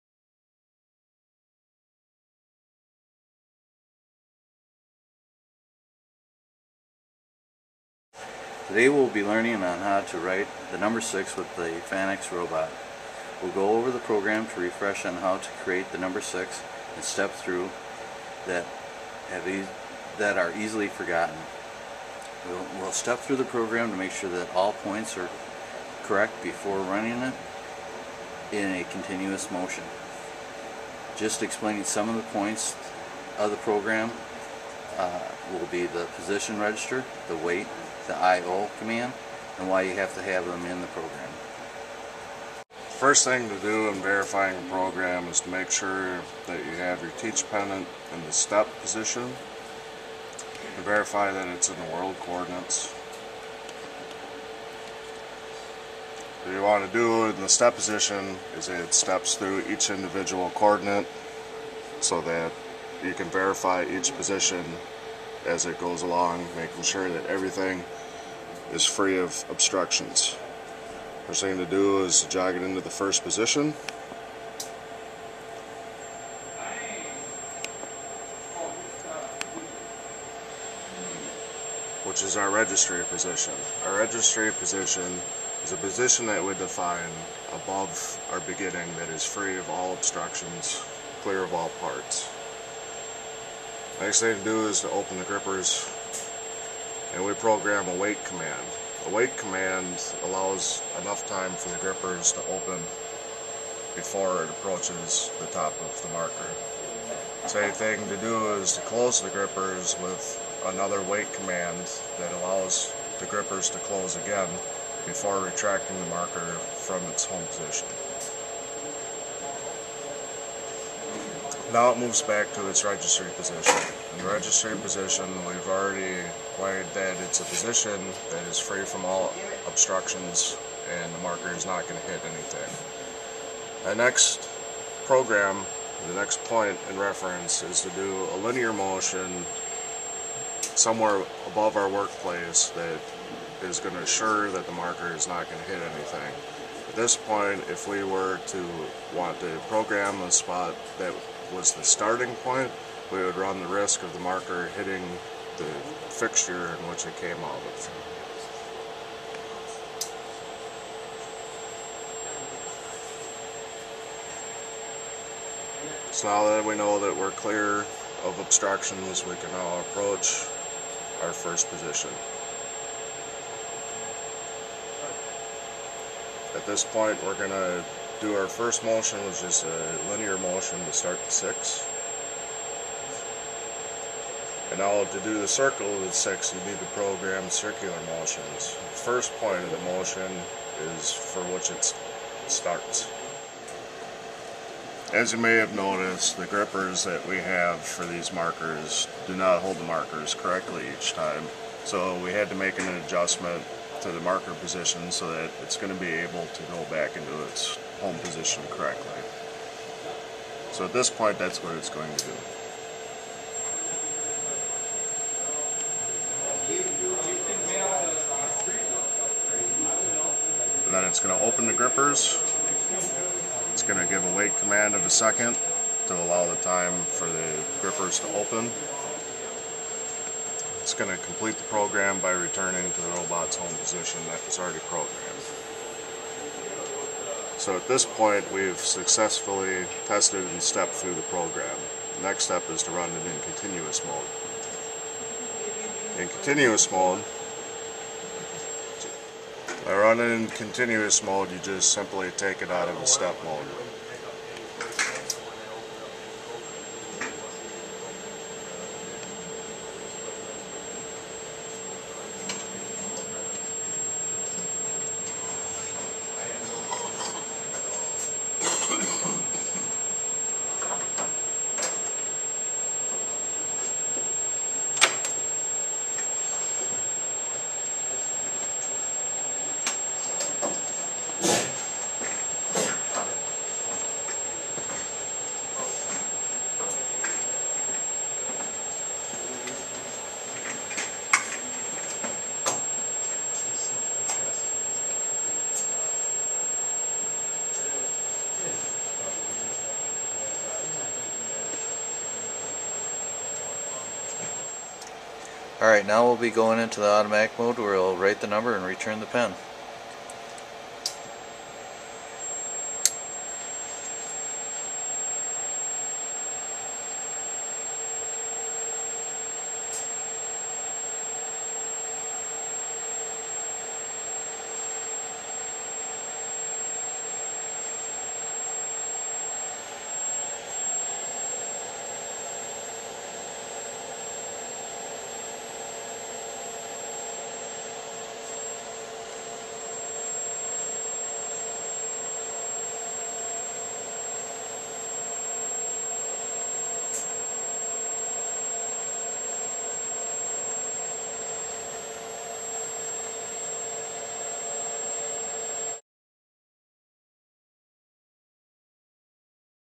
FANUC Teach Pendant programming - Group 1
Students demonstrate in this video how create, teach, test and run a program to pick up a marker, write a number and replace the marker.
Madison Area Technical College offers training in Automated Manufacturing. This training was made possible through a grant from the Economic Development Administration (EDA).
Works was performed on a FANUC Robotics CERT cart with an LRMate 200iC.